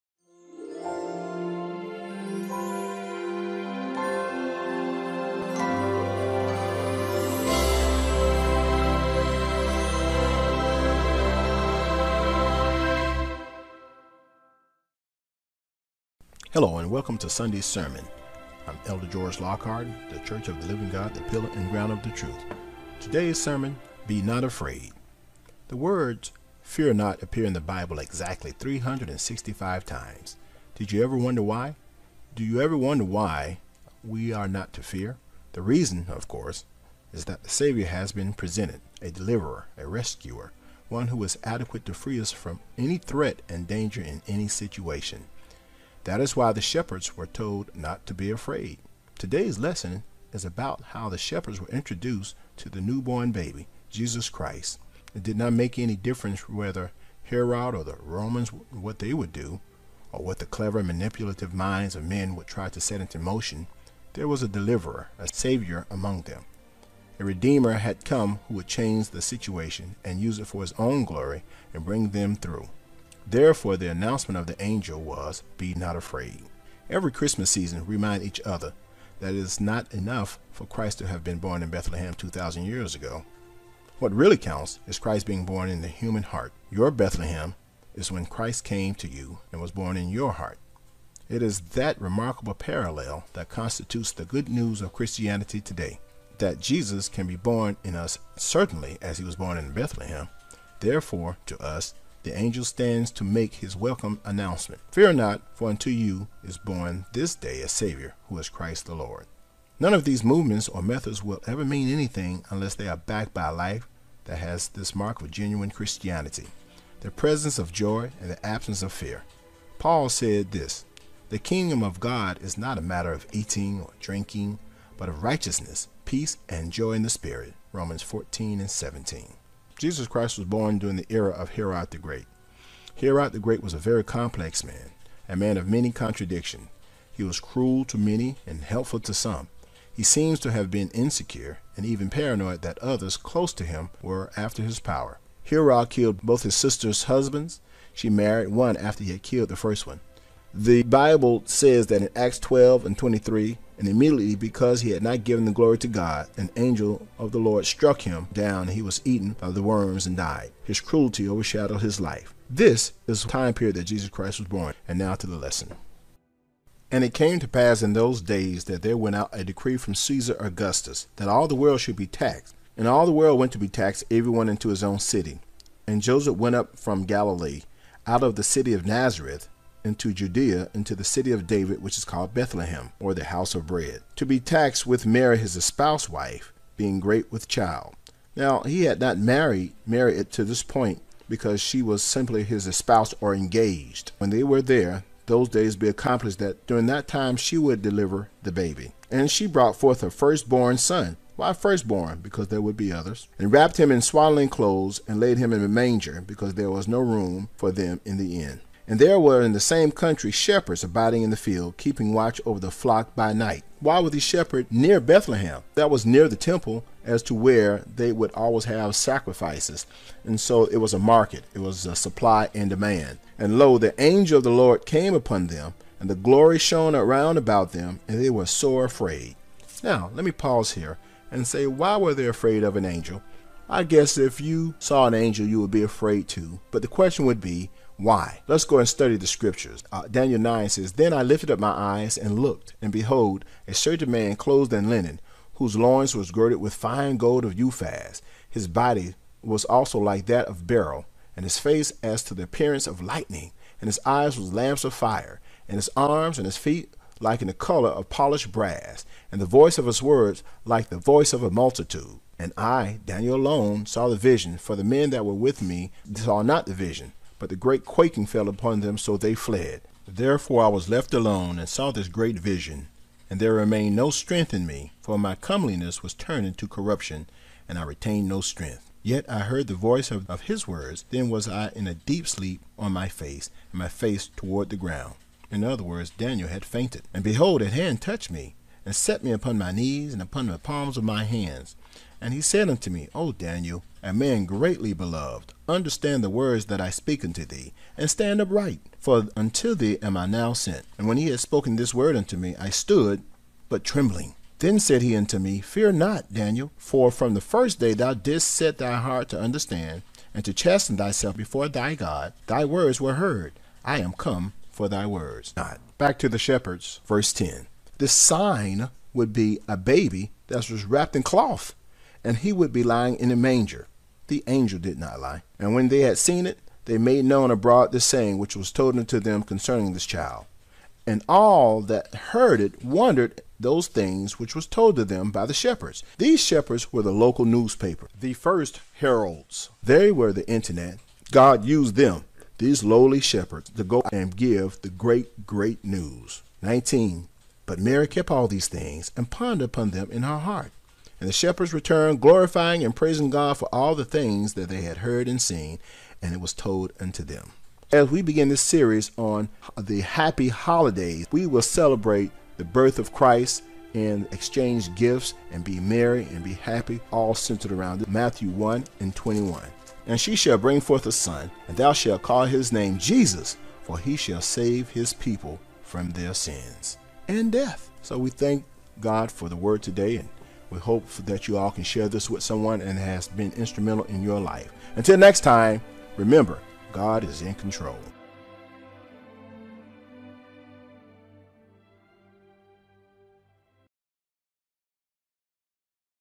Shepherds Angels Annunciation - Be Not Afraid For a Saviour is Come
There were shepherds in the same country staying in the field and keeping watch by night over their flock. And look, an angel of the Lord stood by them, and the glory of the Lord shone around them, and they were terrified.  But, the angel came bearing good tidings. The angel said to them, “Do not be afraid, for see, I bring you good news of great joy which will be to all the people. For there is born to you, this day, in the city of David, a Savior, who is the Messiah, the Lord. This is the sign to you: you will find a baby wrapped in swaddling clothes, lying in a manager.”